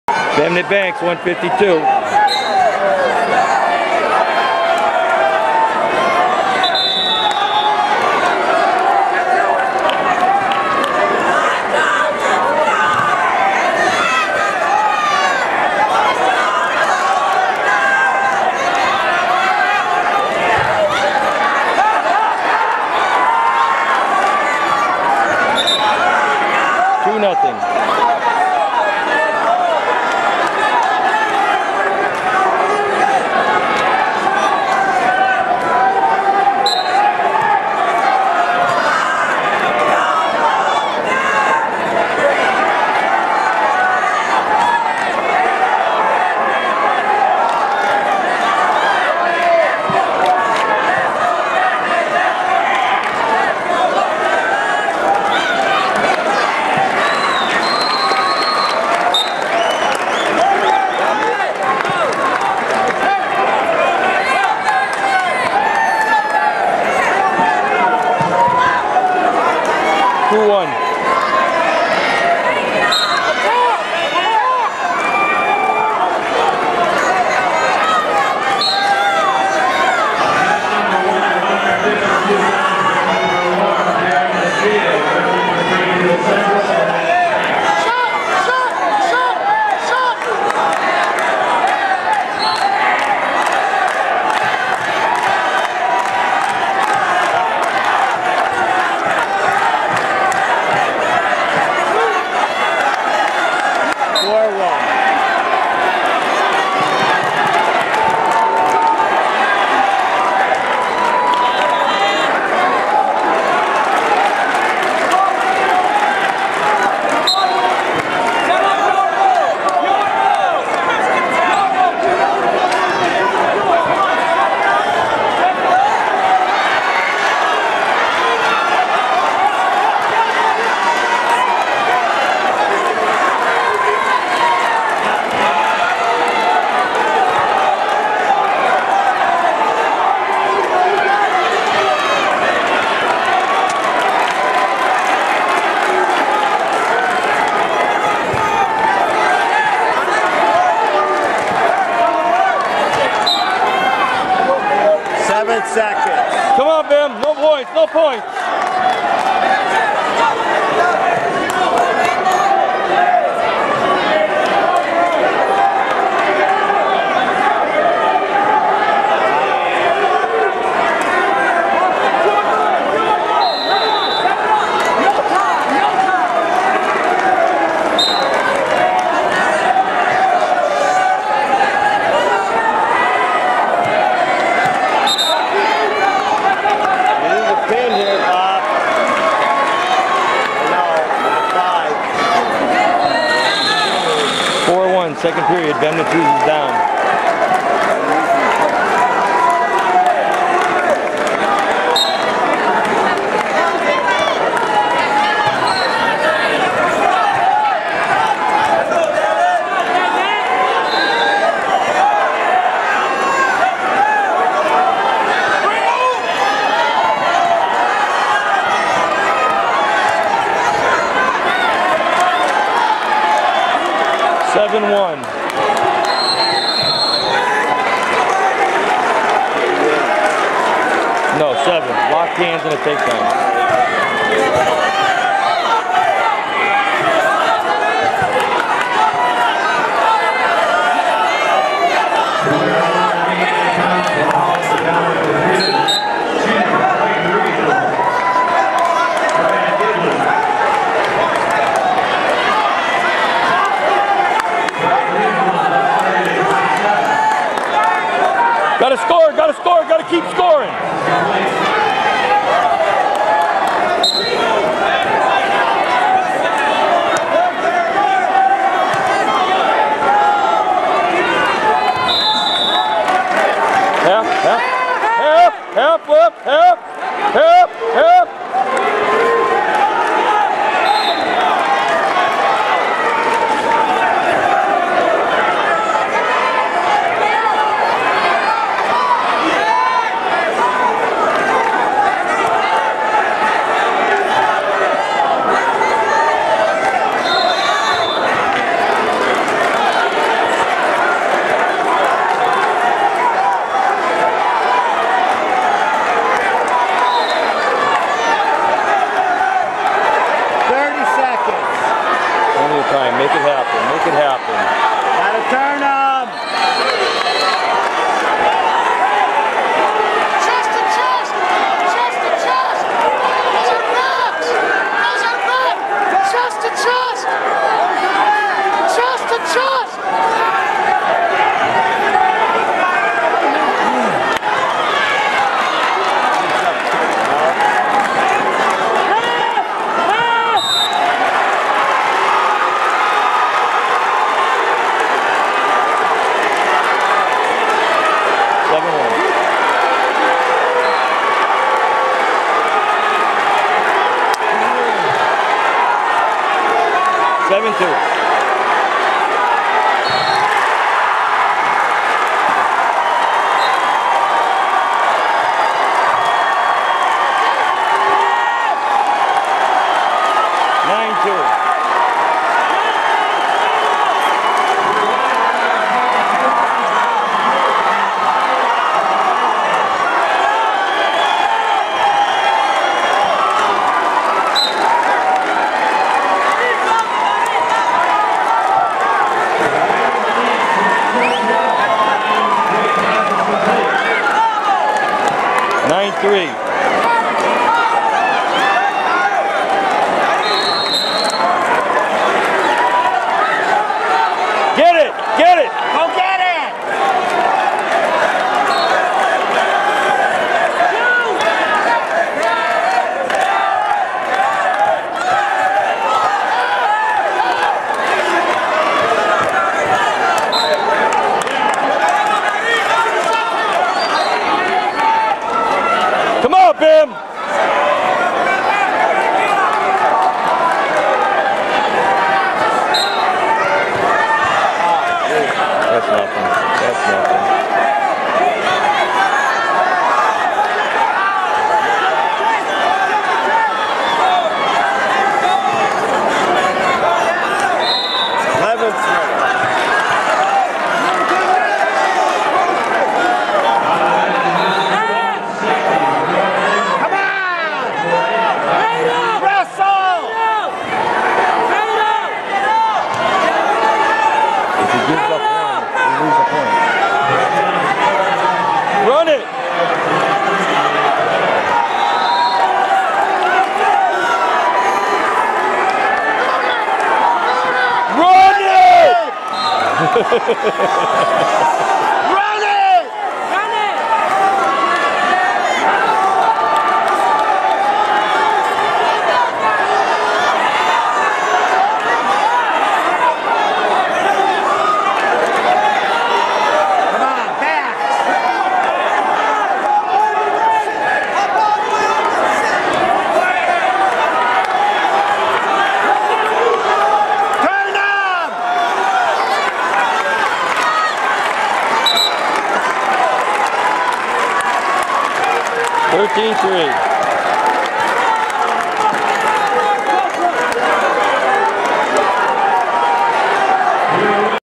152 Banks Lawrence
State D1 Dual meet team Championships at WPI. Central vs Lawrence on February 16, 2011.
152 lb Match, Bemnet Banks (Central) vs Darren Castillo (Lawrence). Banks wins 13-3